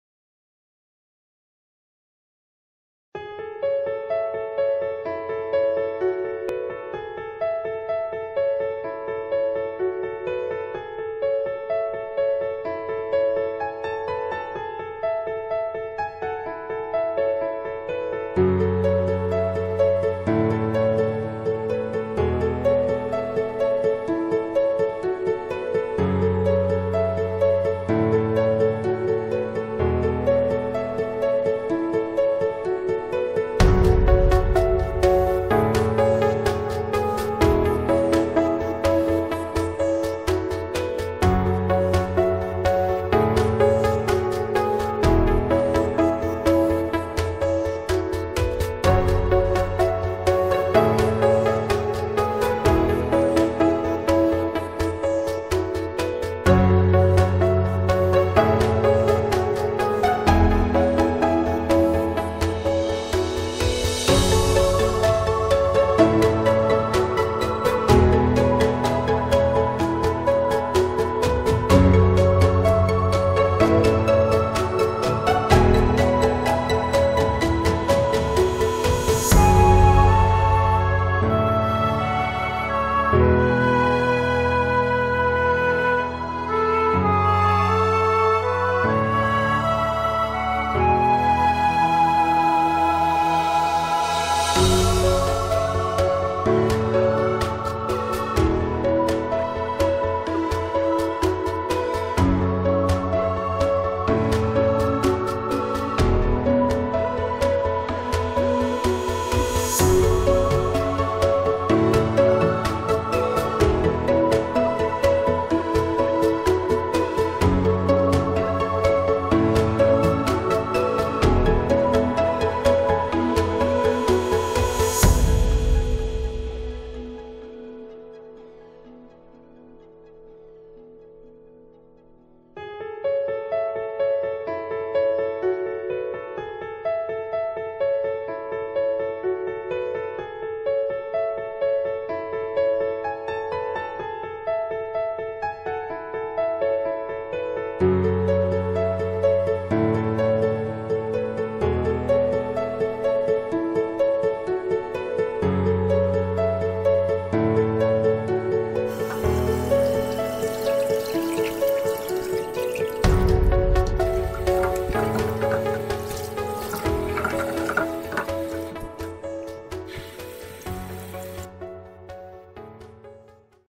Mamibot MULTI-PURPOSE floor cleaner, floor washer, floor vacuum integrated. FLOMO I demo video.
Mamibot's FLOMO I - new product of 2020: Multi-purpose floor washer, floor vacuum, floor scrubber, wet and dry cleaning integrated; Cordless design, no wire needed; Self-cleaning base for roller brush cleaning; Washes all types of hard floor; Dirty water tank and Clean water tank separated; Voice reminding; self-charging base; strong vacuuming power up to 5500PA, up to 30 minutes running time; LED display screen to indicate working status; UVC lamp can kill bacteria in use.